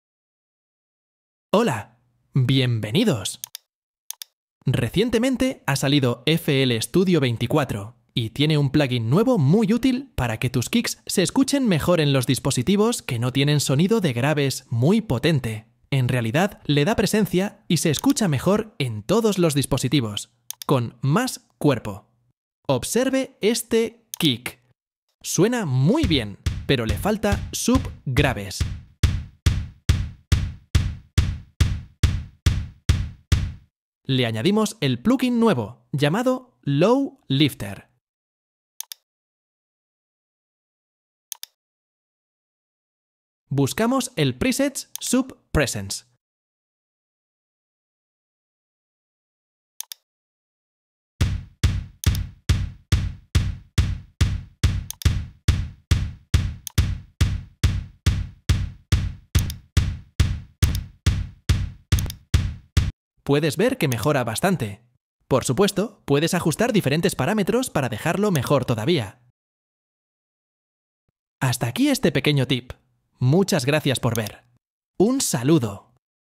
Tip KICK más contundente en FL STUDIO 24 - Armónicos nuevos para tus bombos 🎶
Te enseño un nuevo tip para hacer bombos con más presencia, añadiéndoles nuevos armónicos graves para que suenen mejor, sobre todo en dispositivos móviles y auriculares con poca potencia en los graves.